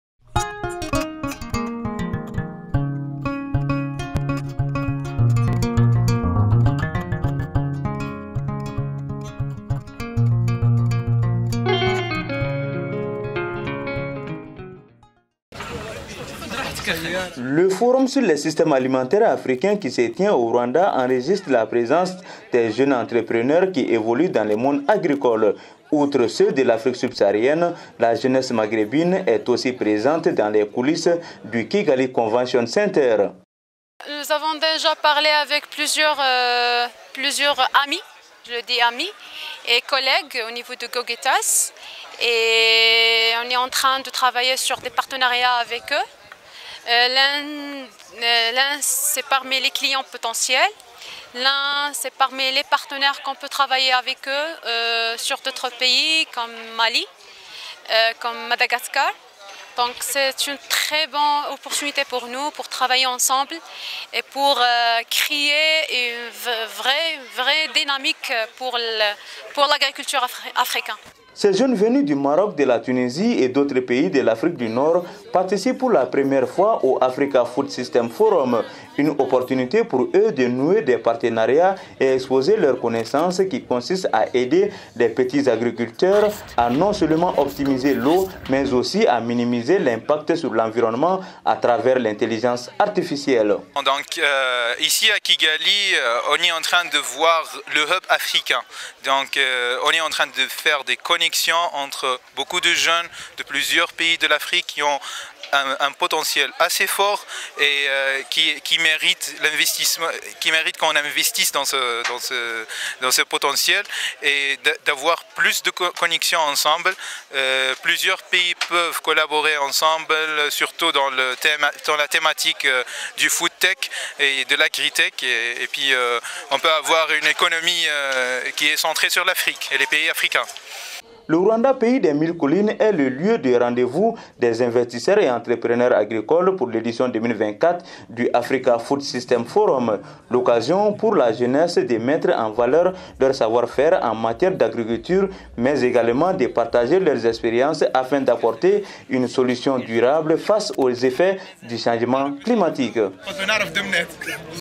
AFSF 2024: La jeunesse du Maghreb à la quête de nouvelles expériences au Africa Food Systems Forum
En plus des jeunes de l'Afrique subsaharienne, la jeunesse de l'Afrique du Nord participe au Africa Food Systems Forum à Kigali pour une première.
Ils sont Marocains, Tunisiens et d'autres nationalités du Maghreb à partager leurs expériences et de découvrir d'autres initiatives dans la chaîne de l'agroalimentaire.

Reportage: Vinabé DENA